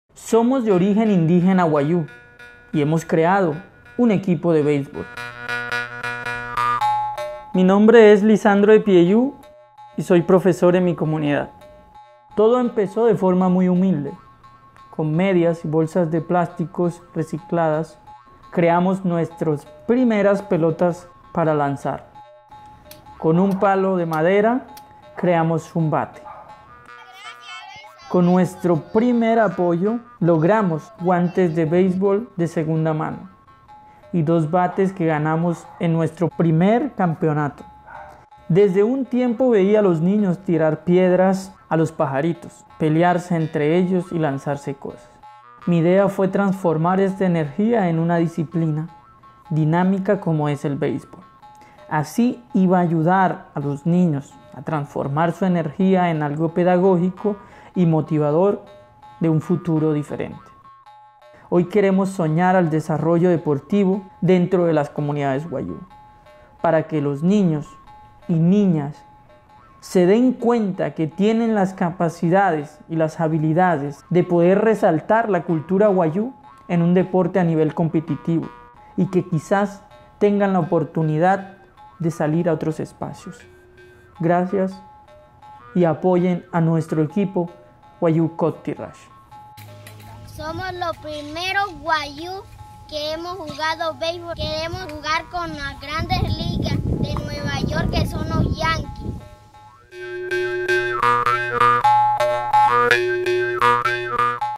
Wayuu people play baseball in Colombia
Lisandro Epiayu, a teacher in the Wayuu community in Colombia, had the idea to start a baseball team: Wayuu Kottirashi. This video tells the story